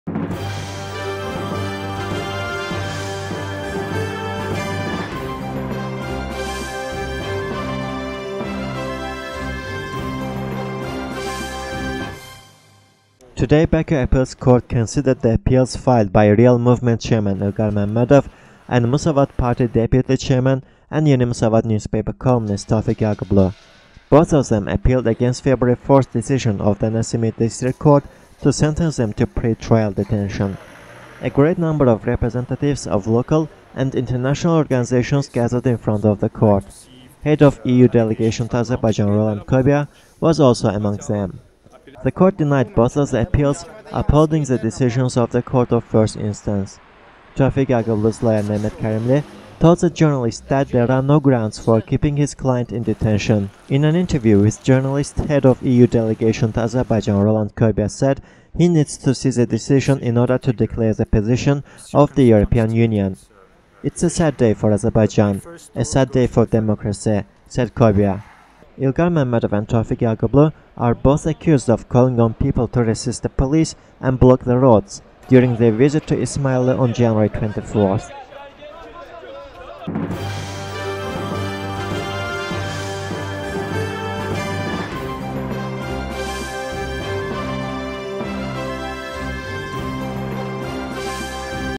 ILGAR MAMMADOV AND TOFIG YAGUBLU TO REMAIN IN PRETRIAL DETENTION
Today Baku Appeals Court considered the appeals filed by REAL Movement chairman Ilgar Mammadov, and Musavat Party deputy chairman and Yeni Musavat newspaper columnist Tofig Yagublu. Both of them appealed against February 4 decision of Nasimi District Court to sentence them to pretrial detention. A great number of representatives of local and international organizations gathered in front of the court. Head of EU Delegation to Azerbaijan, Roland Kobia was also among them.
The Court denied both of the appeals, upholding the decisions of the court of first instance. Tofig Yagublu's lawyer Nemat Karimli told the journalists that there are no grounds for keeping his client in detention.
In an interview with journalists, head of EU Delegation to Azerbaijan Roland Kobia said he needs to see the decision in order to declare the position of the European Union. "It is a sad day for Azerbaijan, a sad day for democracy" said Kobia.
Ilgar Mammadov and Tofig Yagublu are both accused of calling on people to resist the police and block the roads, during their visit to Ismayilli on January 24.